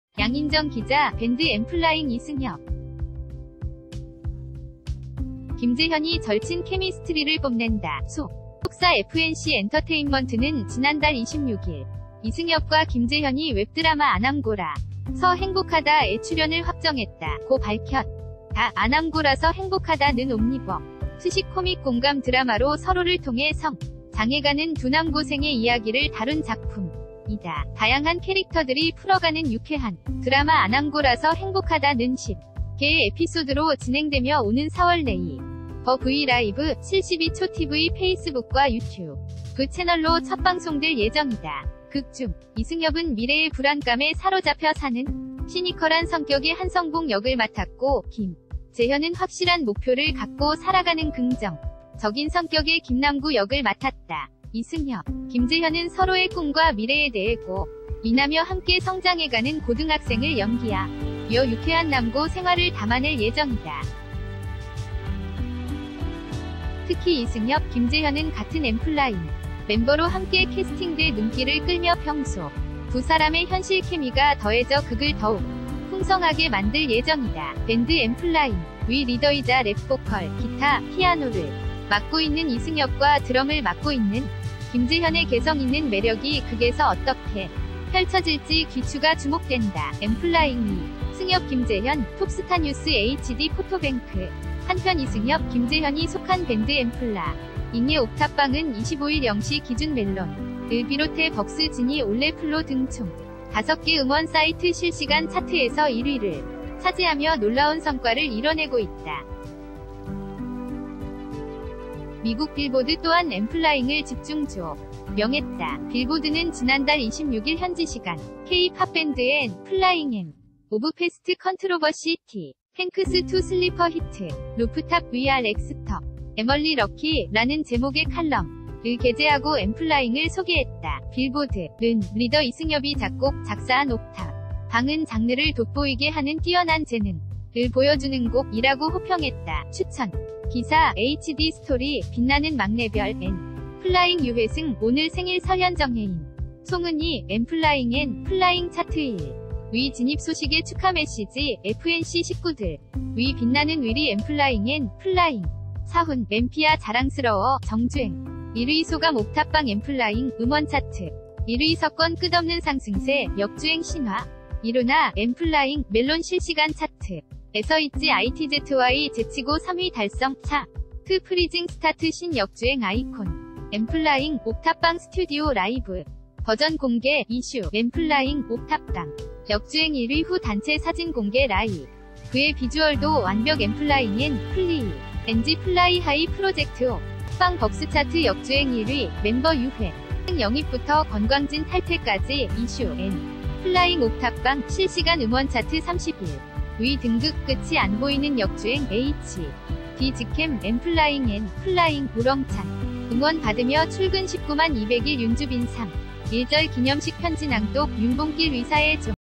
양인정 기자 - 엔플라잉 이승협-김재현 ‘아 남고라서 행복하다’ 캐스팅…현실 케미
[양인정 기자] 밴드 엔플라잉 이승협, 김재현이 절친 케미스트리를 뽐낸다.  소속사 FNC엔터테인먼트는 지난달 26일 “이승협과 김재현이 웹드라마 ‘아 남고라서 행복하다’에 출연을 확정했다”고 밝혔다.  ‘아 남고라서 행복하다’는 옴니버스식 코믹 공감 드라마로 서로를 통해 성장해가는 두 남고생의 이야기를 다룬 작품이다. 다양한 캐릭터들이 풀어가는 유쾌한 드라마 ‘아 남고라서 행복하다’는 10개의 에피소드로 진행되며 오는 4월 네이버 V라이브, 72초tv 페이스북과 유튜브 채널로 첫 방송될 예정이다. 극 중 이승협은 미래의 불안감에 ...